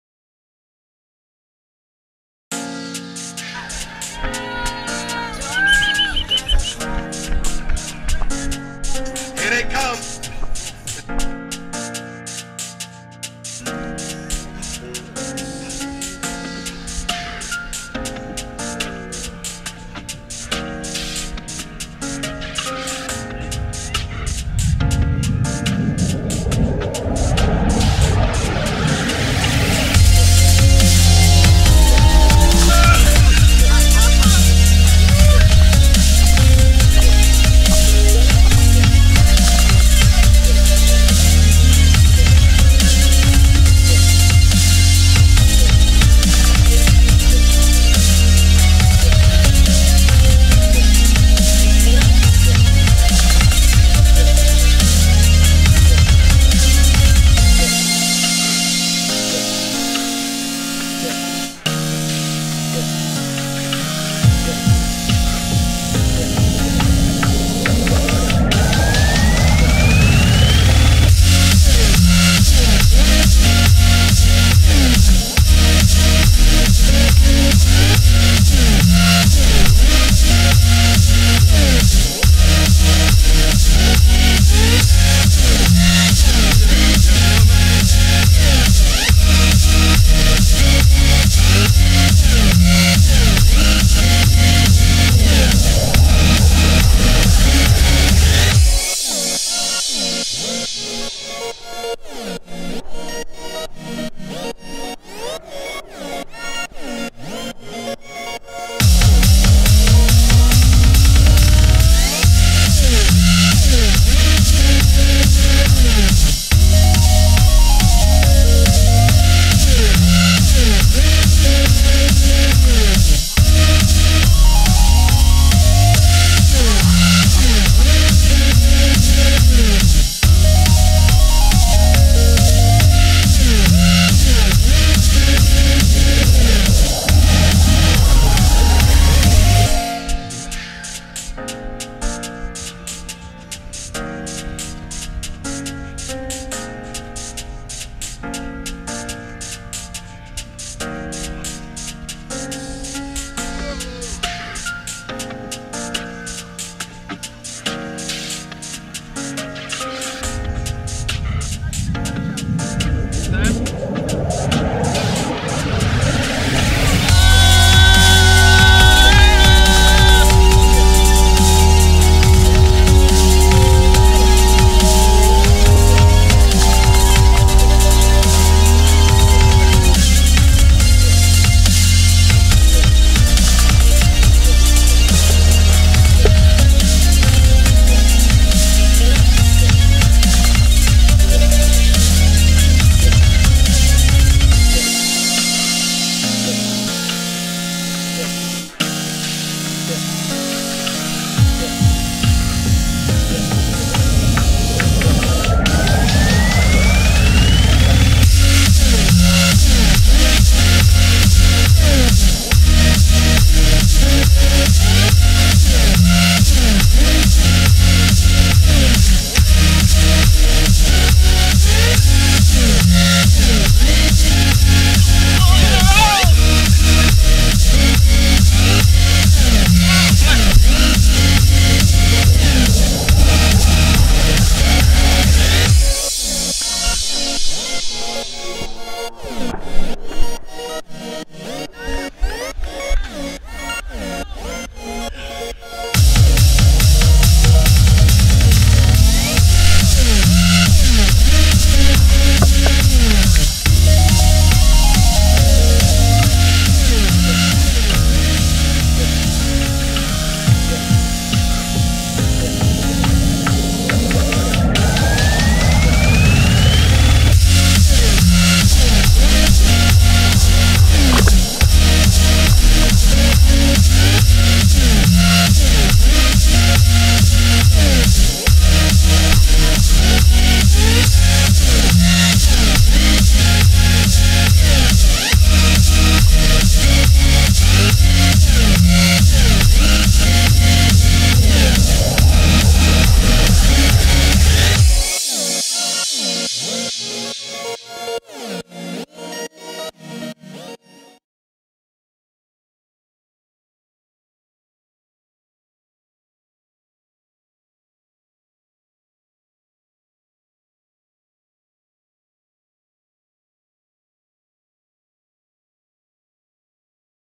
ROC RACE | NYC 2016
ROC Race @ Aviator Sports & Event Center in Brooklyn NY

DATE: Saturday September 10th, 2016

Please comment if you have great music I can use